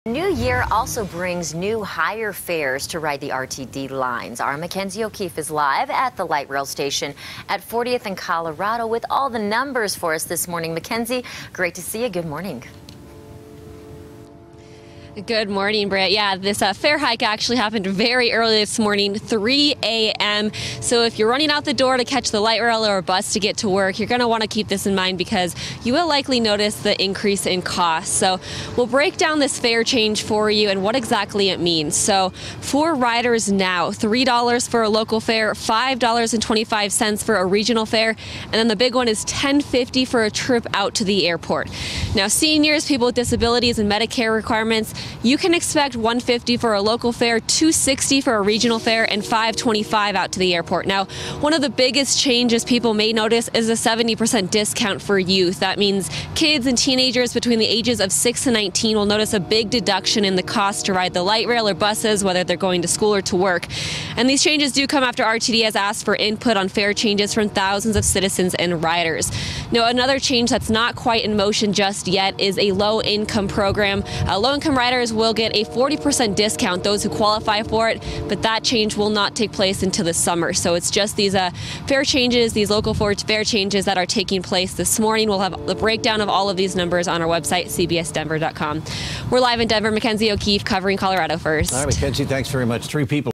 Higher Fares For RTD In Effect For 2019
All light rail, train, bus fares increased but youth fares dropped.